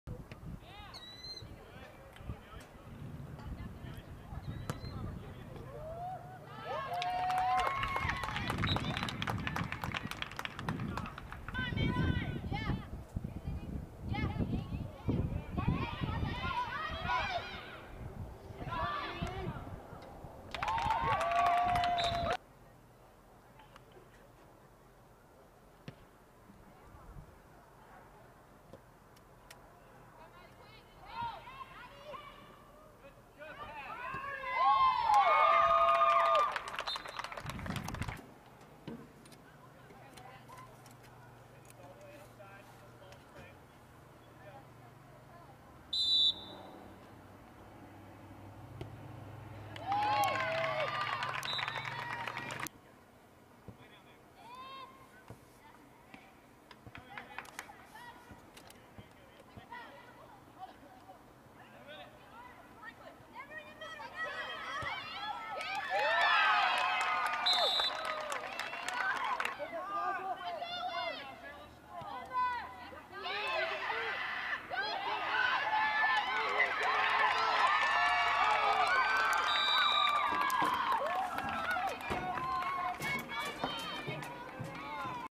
WCHS Soccer- Goals by Lexi Bowen, Toneisha Williams, Alexis Bowen, Emily Faulk and Emma Dippel
WCHS  defeats West Bladen 6-0.